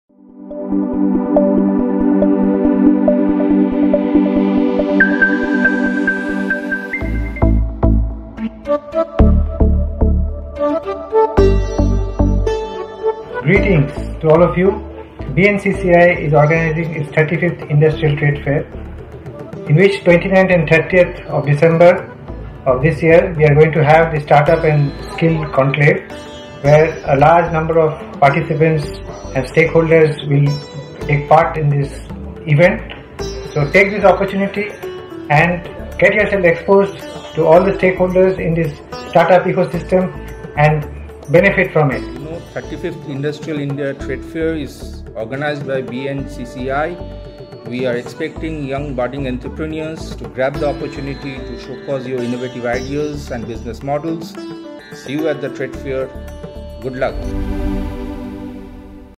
Industrial India Trade Fair, 2022-23 || DaycoIndia
Dear Investor,

We are delighted to invite you to the 35th Industrial India Trade Fair (IITF) held at BNCCI Fair Ground, Newtown from 23 Dec 2022 to 01 Jan 2023. Do visit us at stall 78 to participate in a Financial Awareness Quiz and win attractive investment plans from our SEBI Registered Investment Advisor Division, to suit your needs.

You may also visit our stall for:

✅ Check your Risk Profile
✅ Goal-based Investment Planning
✅ Financial Planning

Dayco is proud to be the Concept Partner of IITF, 2022-23.

Stall Number 78, BNCCI Fair Ground (Near Coffee House New Town),  Plot No. IIA/13, Action Area IIA/13, Premises No. 17-0374, Rajarhat, New Town, Kolkata

🕐 Timing: 1 pm – 7 pm
🗓️ Dates: 23rd Dec 2022 to 1st Jan 2023